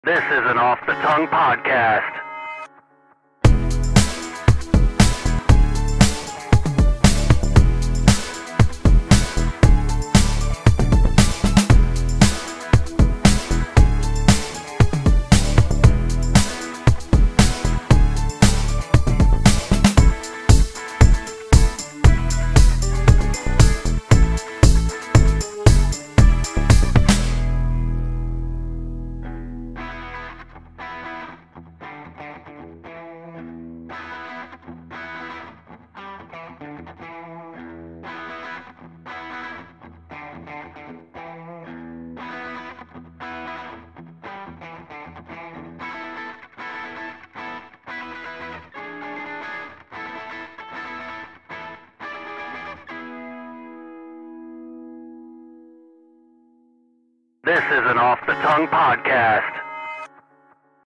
Photo Shoot Short
Had a blast doing a photo shoot for the upcoming online magazine interview we’re doing for the OFF THE TO GUE PODCAST NETWORK!  Don’t forget us when we’re famous!